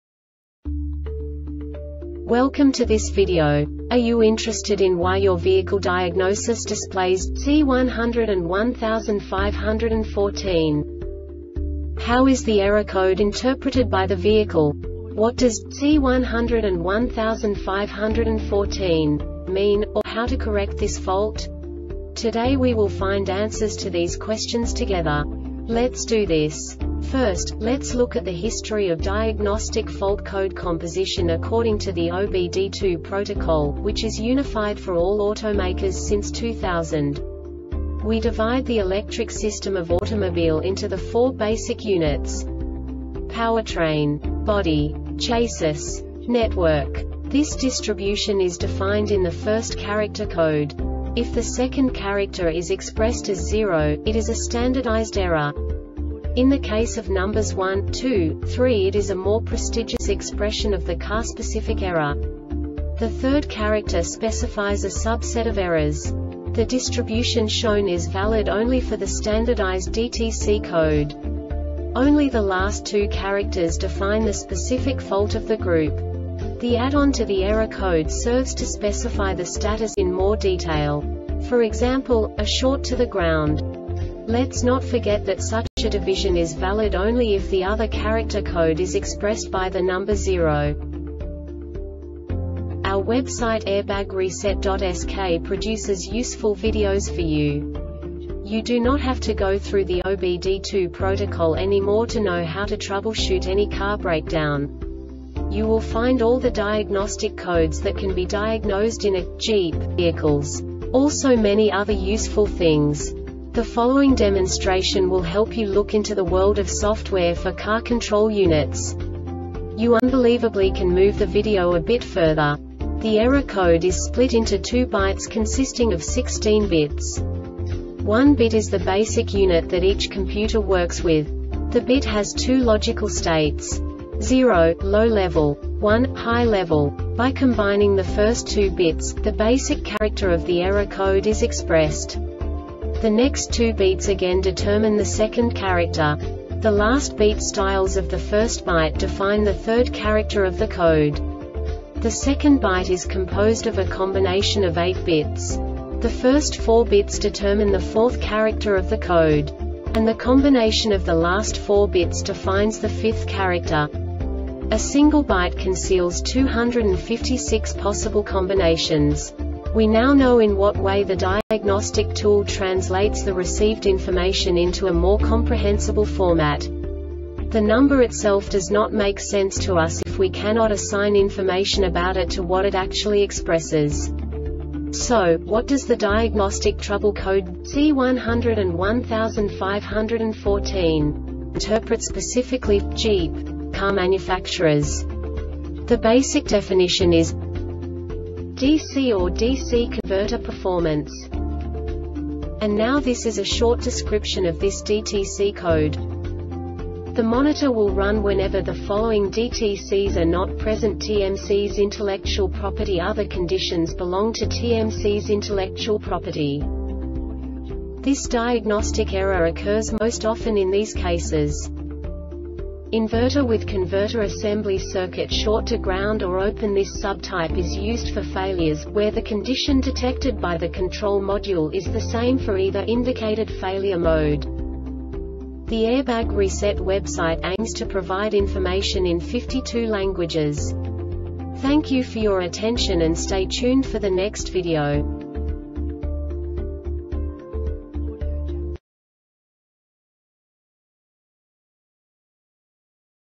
DTC Jeep C1015-14 Short Explanation
Contents:
0:21 Basic DTC analysis according to OBD2 protocol standard.
1:48 Insight into programming
2:58 A diagnostically understandable description of the Diagnostic Trouble Code
3:05 Basic error definition

Make:
Jeep
Code:
C1015 14
Definition:
DCorDC Converter Performance
Description:
The monitor will run whenever the following DTCs are not present: TMCs intellectual property Other conditions belong to TMCs intellectual property: -
Cause:
 Inverter with converter assembly
Failure Type: